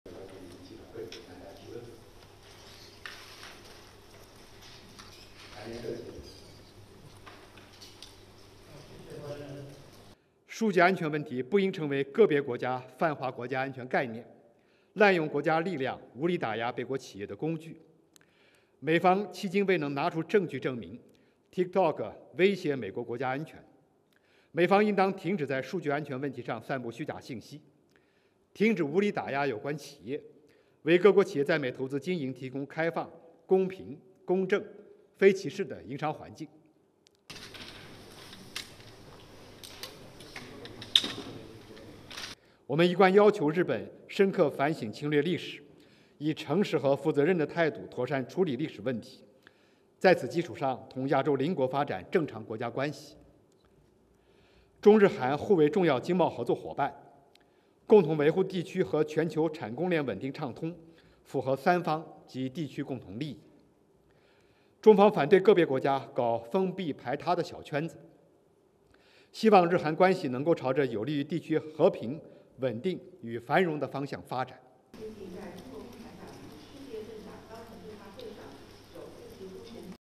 China comments on TikTok and Japan-SKorea
RESTRICTION SUMMARY:

ASSOCIATED PRESS
Beijing - 16 March 2023
1. Wide of news conference
2. Journalists
3. SOUNDBITE (Mandarin) Wang Wenbin, Chinese Foreign Ministry spokesperson:
"Data security should not be used by a certain country to abuse the national security concept and state power to suppress foreign companies. The U.S. has yet to present evidence that TikTok threatens its national security. The U.S. should stop spreading disinformation about data security, stop unreasonably suppressing the relevant company and provide an open, fair and non-discriminatory environment for foreign businesses to invest and operate in the U.S."
4. Journalists
5. SOUNDBITE (Mandarin) Wang Wenbin, Chinese Foreign Ministry spokesperson:
"We have always asked Japan to reflect deeply on its history of aggression, deal with historical issues in an honest and responsible manner, and develop normal state-to-state relations with its Asian neighbors on this basis. China, Japan and the ROK (South Korea) are important economic and trade partners and it is in the common interest of the three parties and the region to jointly maintain the stability and smooth flow of the regional and global industrial and supply chain. China opposes putting up exclusive small circles of individual countries. We hope that Japan-ROK relations will develop in the direction of regional peace, stability and prosperity."
6. News conference
STORYLINE:
China on Thursday accused the U.S. of spreading disinformation and suppressing TikTok following reports the Biden administration was calling for its Chinese owners to sell their stakes in the popular video-sharing app.

"The U.S. has yet to present evidence that TikTok threatens its national security," Foreign Ministry spokesperson Wang Wenbin told reporters at a daily briefing in Beijing.

TikTok was dismissive on Wednesday of reports that the Biden administration was calling for its Chinese owners to sell their stakes in the popular video-sharing app, saying such a move wouldn’t help protect national security.

The company was responding to a report in The Wall Street Journal that said the Committee on Foreign Investment in the U.S., part of the Treasury Department, was threatening a U.S. ban on the app unless its owners, Beijing-based ByteDance Ltd., divested.

Late last month, the White House gave all federal agencies 30 days to wipe TikTok off all government devices.

Some agencies, including the Departments of Defense, Homeland Security and State, already have restrictions in place.

The White House already does not allow TikTok on its devices.

Wang also commented on a meeting between Japanese and South Korean leaders on Thursday, saying that China hoped "Japan-ROK relations will develop in the direction of regional peace, stability and prosperity.”

It is in the "common interest" of China, Japan and South Korea to “jointly maintain the stability and smooth flow of the regional and global industrial and supply chain," said Wang.

He added that China rejected what it described as “exclusive small circles.”

Japan and South Korea agreed on steps to resolve a trade dispute hours before a highly anticipated summit began on Thursday, a sign that the two countries are making concrete progress on their bid to overcome disputes over history and rebuild their nations’ security and economic ties.